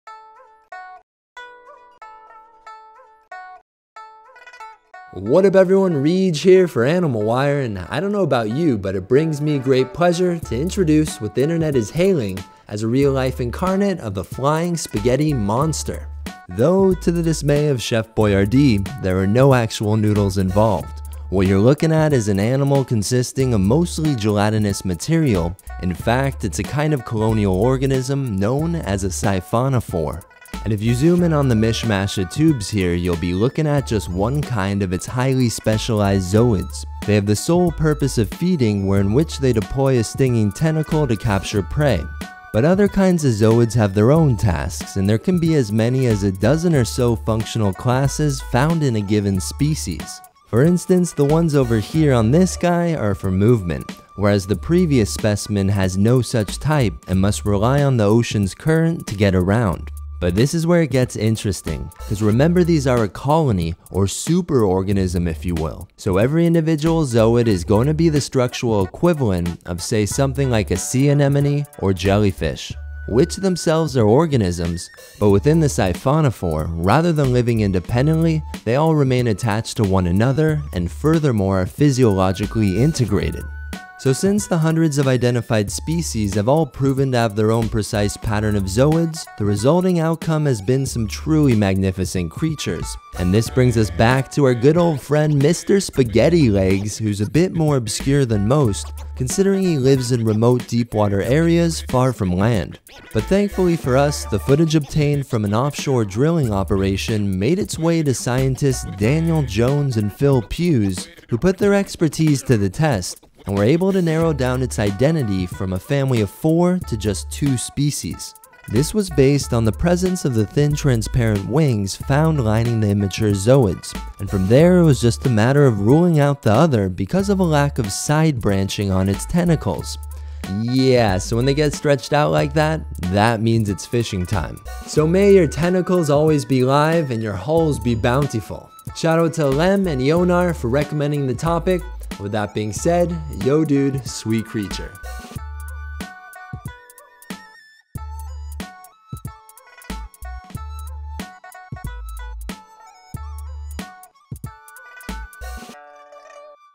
FLYING SPAGHETTI MONSTER SEA CREATURE
The legends are true! It looks like the flying spaghetti monster does really exist…or at least something very similar looking. Check out this rare siphonophore that was discovered through a collaborative effort known as the SERPENT Project.

Courtesy of:
Daniel Jones
National Oceanography Centre
BP Exploration (Angola) Limited
Sonangol

Supporting Media...coming soon.